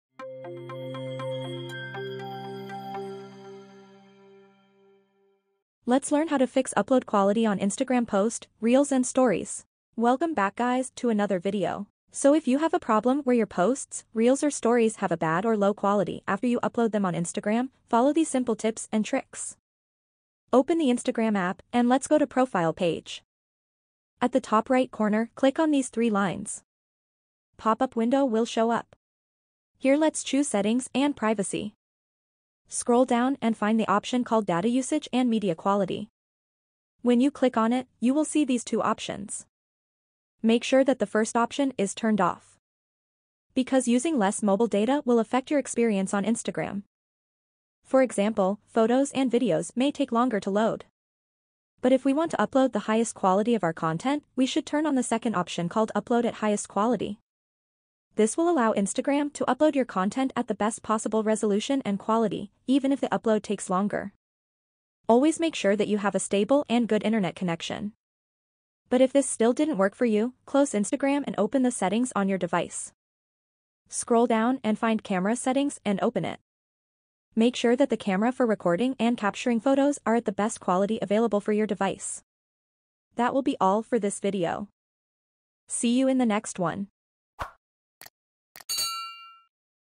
Fix Instagram Story Bad Video Quality After Upload Highest Quality Uploads Instagram 2023
Let’s learn how to fix the problem with bad quality uploads after posting. If you have a problem with photos, videos, Instagram Reels or Stories where they have a low quality or low resolution, then this video is for you!